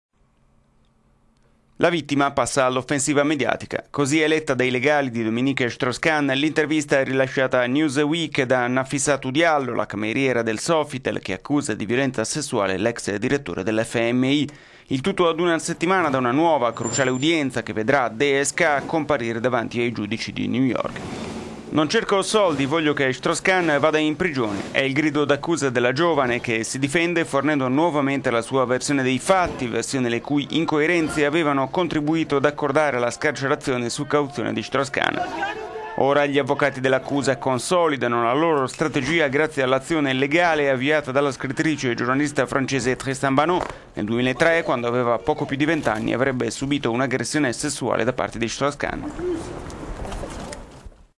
Strauss-Kahn: cameriera parla a 'Newsweek'
La vittima passa all'offensiva mediatica. Così è letta dai legali di Dominique Strauss-Kahn l'intervista rilasciata a 'Newsweek' da Nafissatou Diallo, la cameriera del Sofitel che accusa di vioenza sessuale l'ex Direttore dell'Fmi. Il tutto ad una settimana da una nuova, cruciale udienza che vedrà DSK comparire davanti ai giudici di New York.